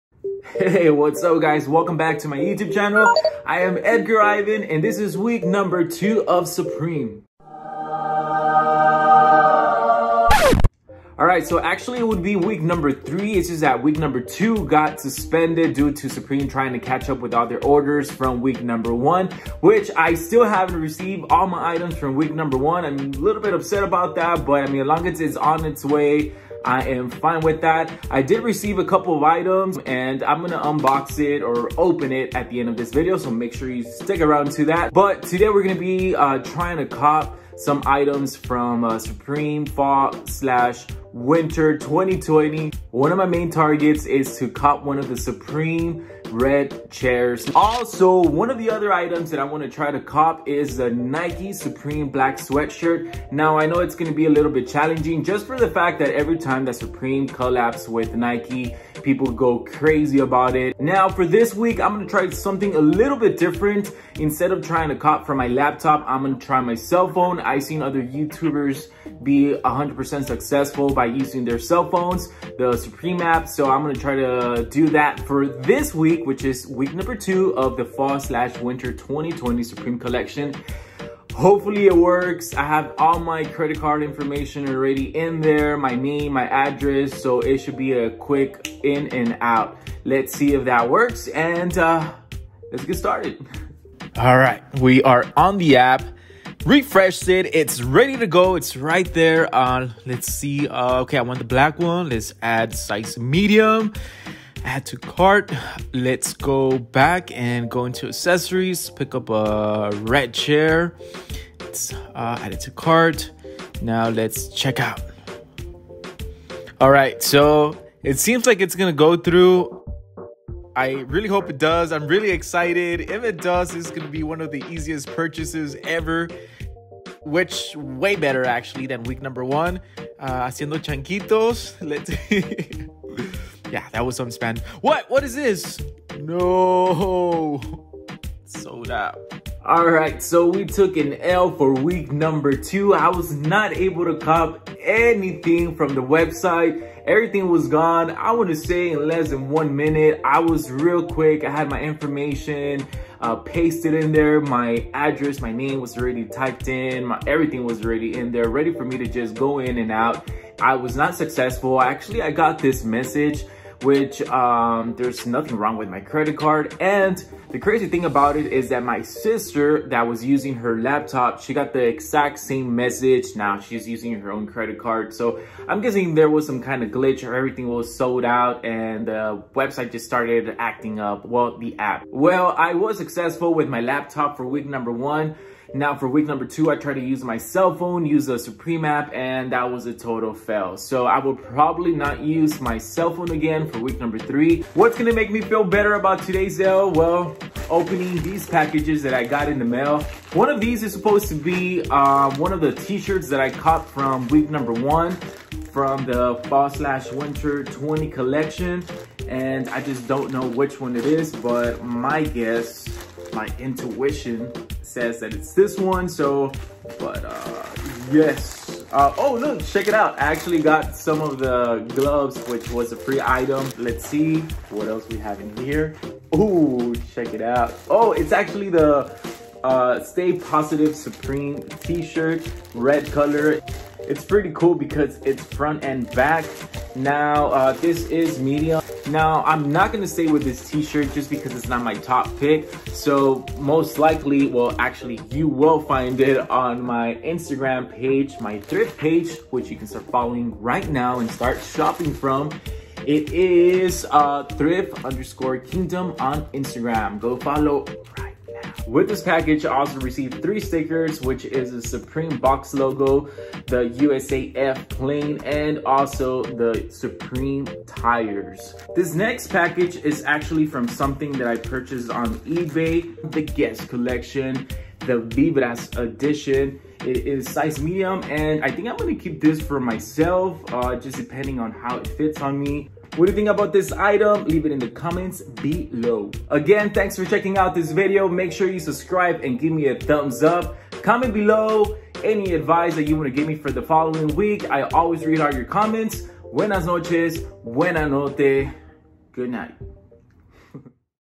SUPREME NIKE DROP - Supreme FW20 Week 2 Live Cop ON THE APP
On a mission to cop supreme by using the app, week 2 FW20 includes the Nike Collection.